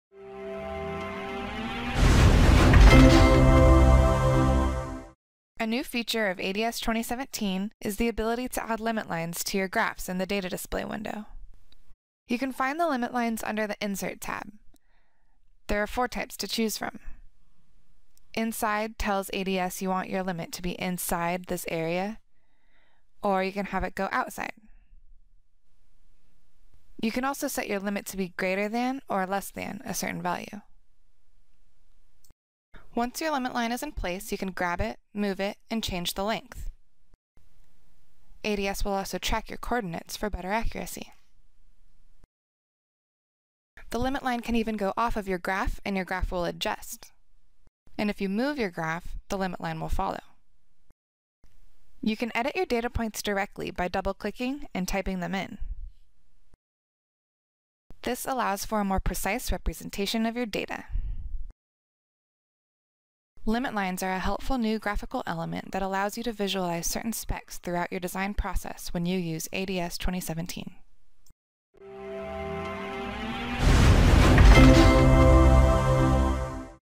ADS Data Display Limit Lines
Quickly see simulation results versus design goals in the data display window. Advanced Design System (ADS) 2017 now has the ability to add limit lines in your graphs. You can set your limit to a specific value or an area. Limit lines are a helpful new graphical element of ADS 2017 which allows you to visualize certain specs throughout your design process.